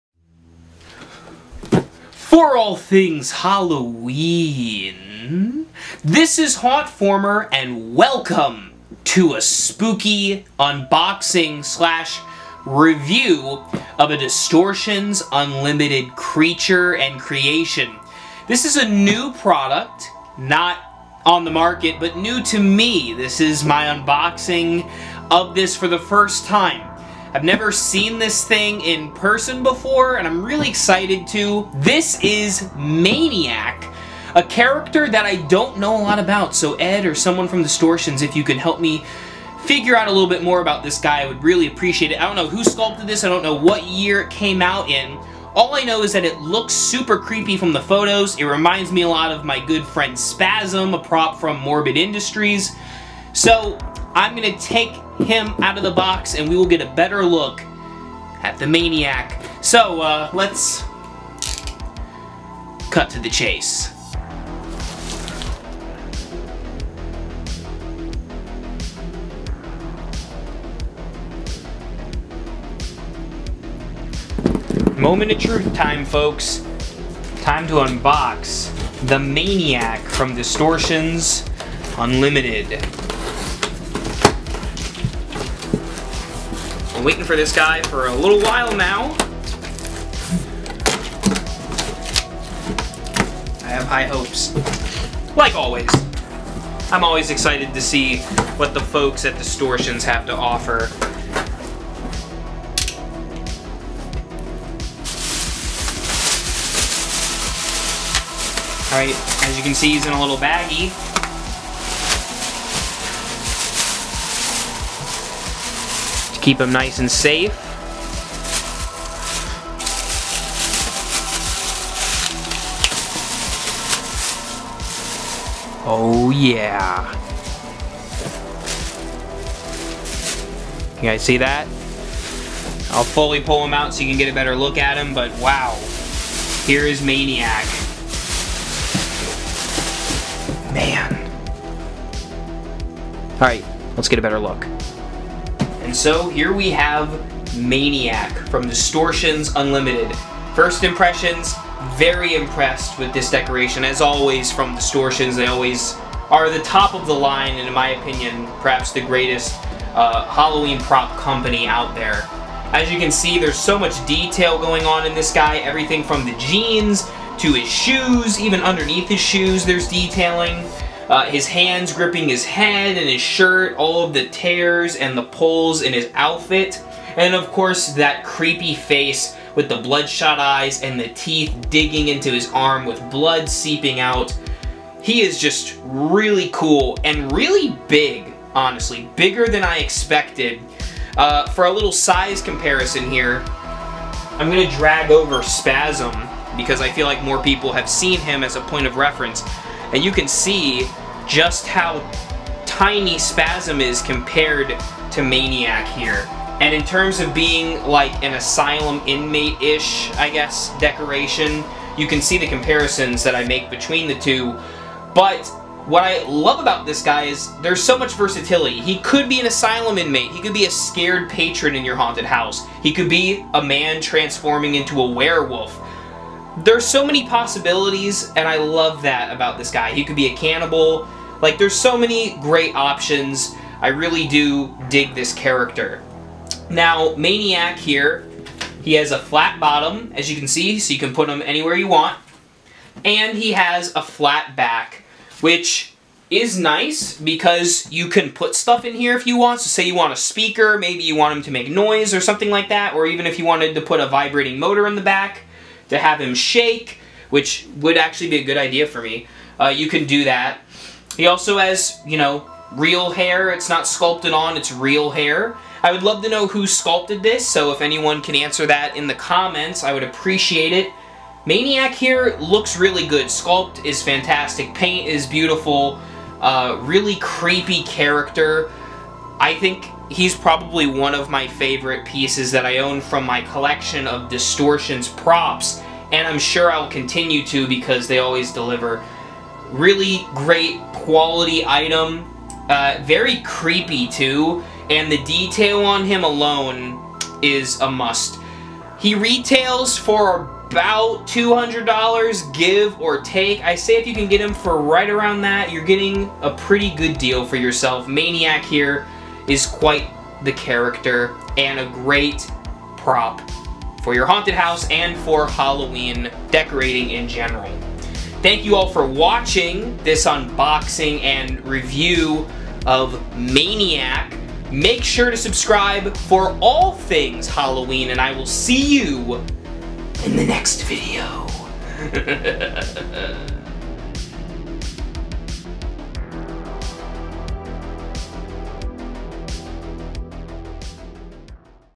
Distortions Unlimited MANIAC Halloween Prop UNBOXING + REVIEW
From the walls of Distortions comes this maniac dubbed, well, MANIAC! A life-sized foam-filled Halloween decoration.